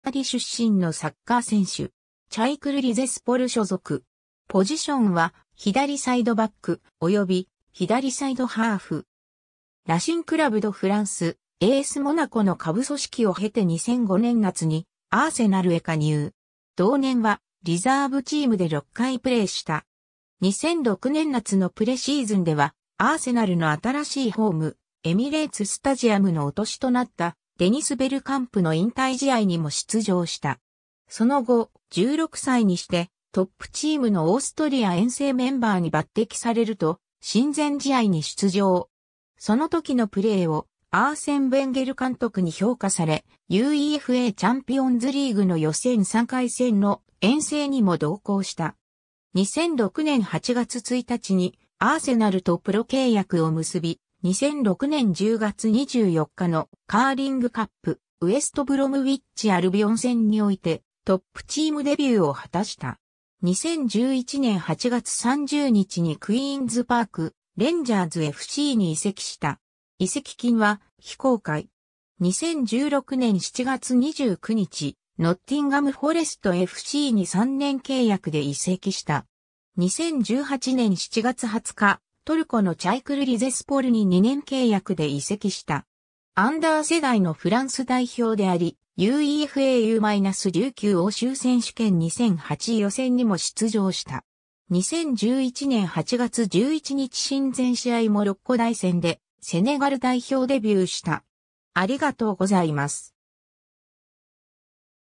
アルマン・トラオレ
カーディフ・シティFCの選手
チャイクル・リゼスポルの選手
アルマン・トラオレ（Armand Traoré, 1989年10月8日 - ）は、フランス・パリ出身のサッカー選手。
チャイクル・リゼスポル所属。
ポジションは左サイドバック、及び左サイドハーフ。
 ラシン・クラブ・ド・フランス、ASモナコの下部組織を経て2005年夏にアーセナルへ加入。
同年はリザーブチームで6回プレーした。
 2006年夏のプレシーズンでは、アーセナルの新しいホーム、エミレーツ・スタジアムの杮落としとなったデニス・ベルカンプの引退試合（対アヤックス）にも出場した。
その後、16歳にしてトップチームのオーストリア遠征メンバーに抜擢されると、親善試合（対AZアルクマール、対SVマッテルスブルク）に出場。
その時のプレーをアーセン・ベンゲル監督に評価され、UEFAチャンピオンズリーグの予選3回戦（対ディナモ・ザグレブ（クロアチア））の遠征にも同行した。
 2006年8月1日にアーセナルとプロ契約を結び、2006年10月24日のカーリングカップ、ウェスト・ブロムウィッチ・アルビオン戦においてトップチームデビューを果たした。
 2011年8月30日にクイーンズ・パーク・レンジャーズFCに移籍した。
移籍金は非公開。
 2016年7月29日、ノッティンガム・フォレストFCに3年契約で移籍した。
 2018年7月20日、トルコのチャイクル・リゼスポルに2年契約で移籍した。
 アンダー世代のフランス代表であり、UEFA U-19欧州選手権2008予選にも出場した。
 2011年8月11日親善試合モロッコ代戦でセネガル代表デビューした。